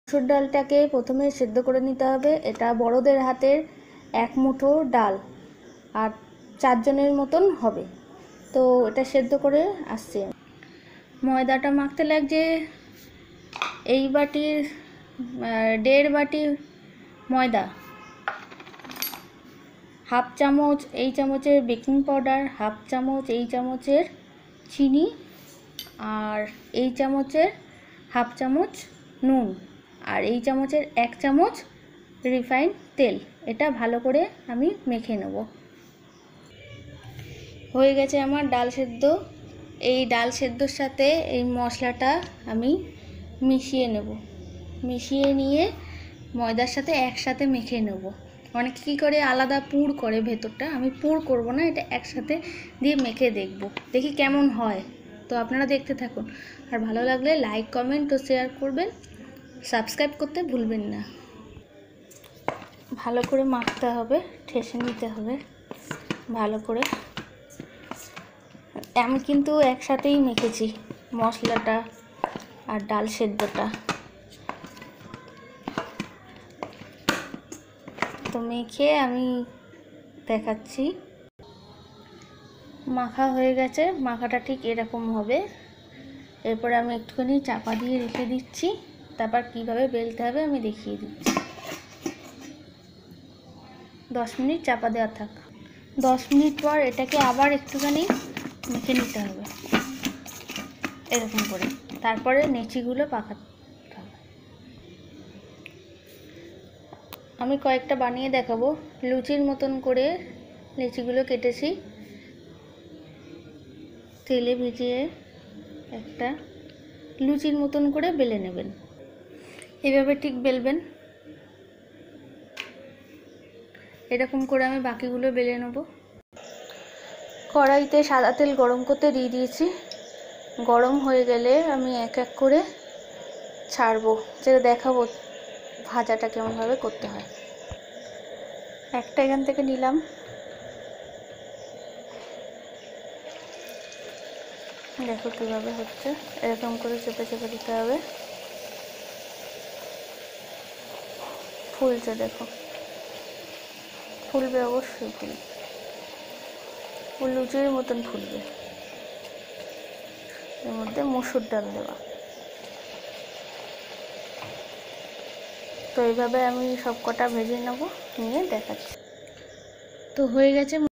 Dalpuri Making with Musur Dal || Musur dal diye Dalpuri || Made by Urmi
Musur dal diye dalpuri ek suswadu ghoroya recipe. Musur dal pur na diye Ekebare siddho kore dow ar moddhe mekhe kora hoy.